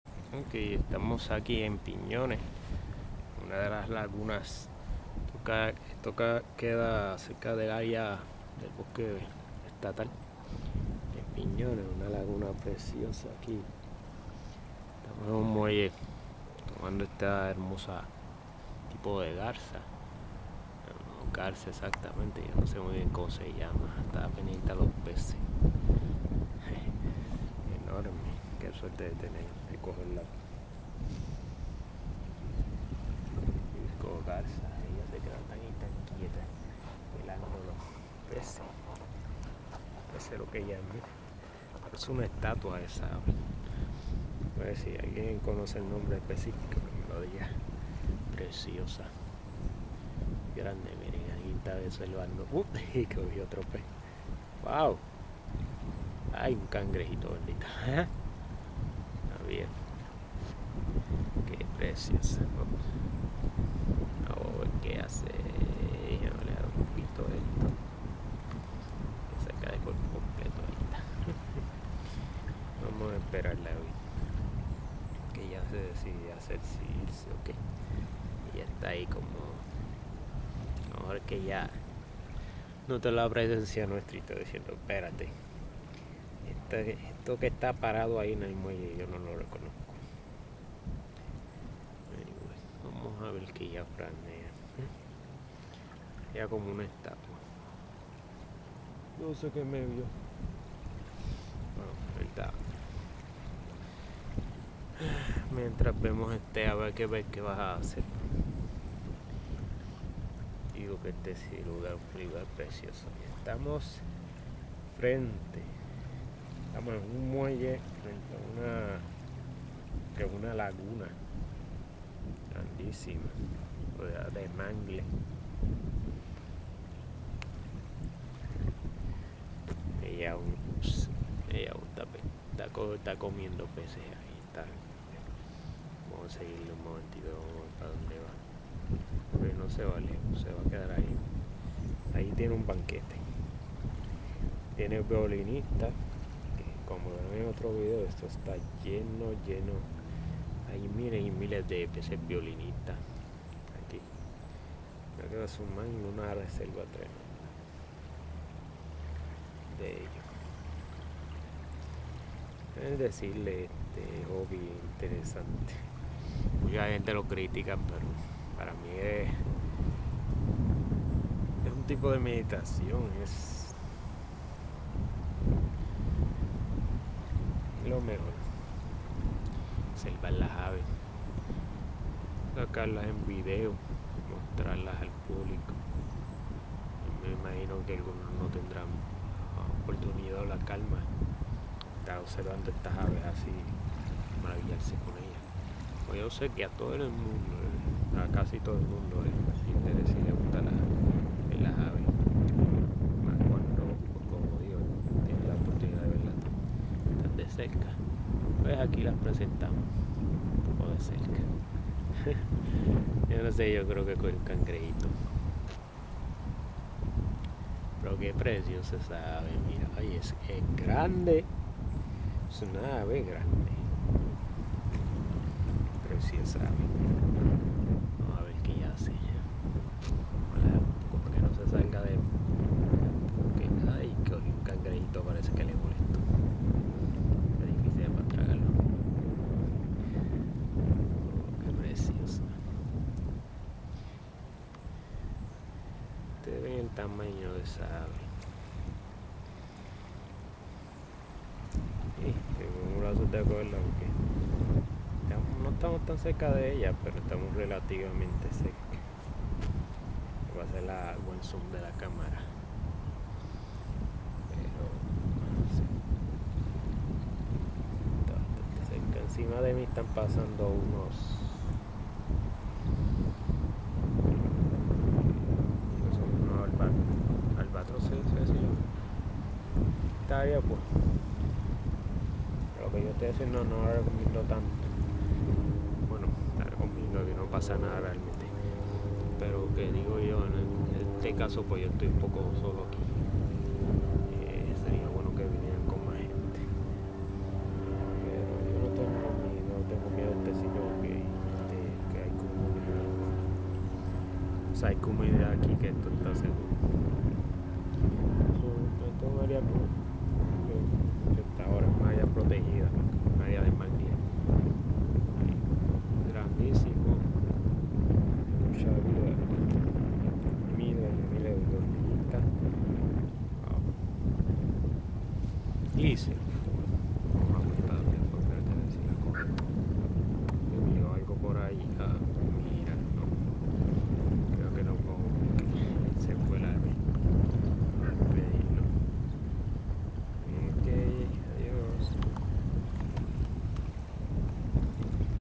Black-Cowned Night Heron en piñones, PR.
Mechas vecez  he visto esta ave, pero nunca tube la oportunidad de gravarla; y me gustaria saber el nombre si alguien lo sabe. Como el titulo suguiere , este video fue tomado en el bosque estatal de piñones. Me sarprendio la cantidad de cangrejos violinistas que se encuentran en el area. Esta es una de las lagunas internas del bosque. Espero que como yo aprecien este video.